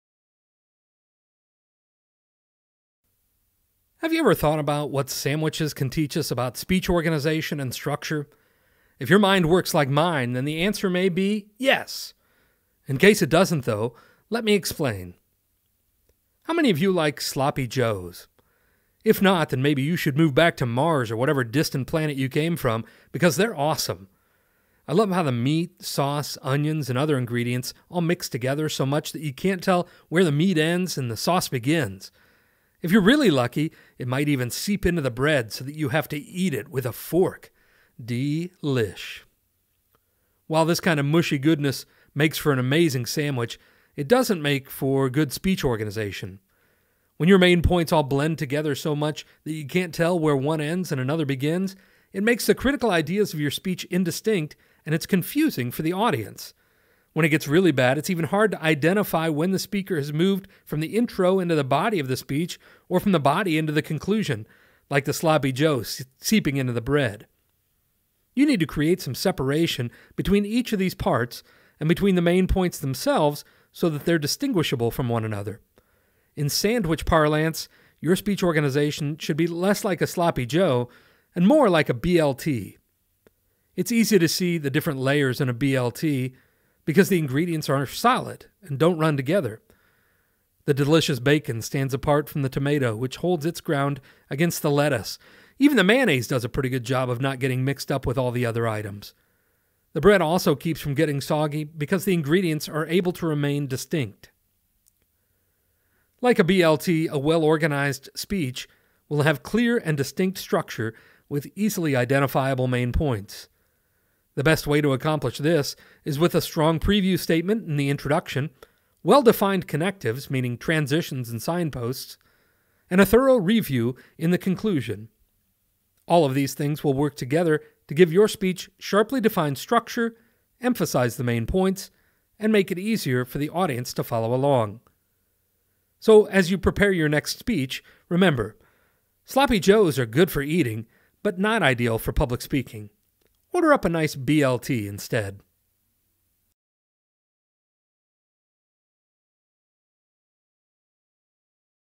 Getting Hungry for Good Speech Organization
A brief analogy to highlight the importance of good organization and structure in public speaking.